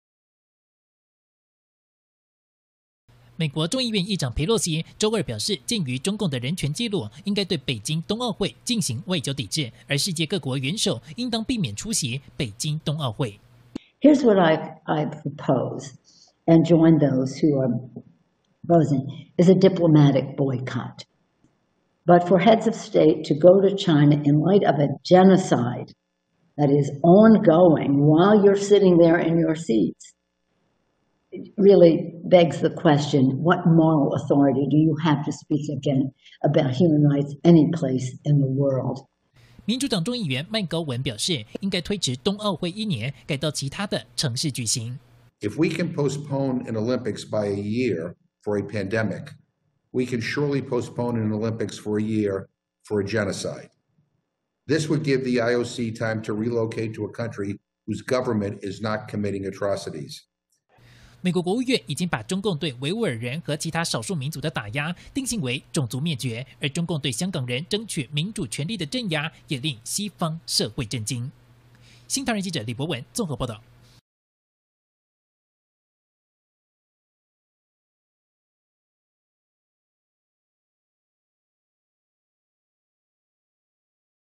美議長裴洛西：籲對北京冬奧「外交抵制」｜@ChinaBreakingNewsTV ｜20210520
美國國會眾議院議長裴洛西，18日呼籲對2022北京冬季奧運會實行「外交抵制」。裴洛西指出，中共踐踏人權，世界各國領導人如果出席北京冬奧會，將會失去他們的道德權威。